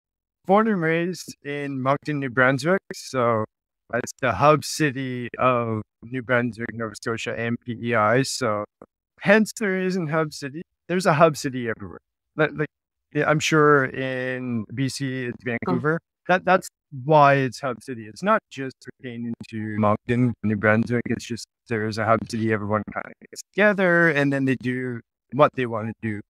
Hub City Overland: Canada Maritimes Four Wheeling In A Toyota Tacoma 4x4 Promo
Full episode Audio at:
Our quick promo for our interview with Ryan at @HubCityOverlanding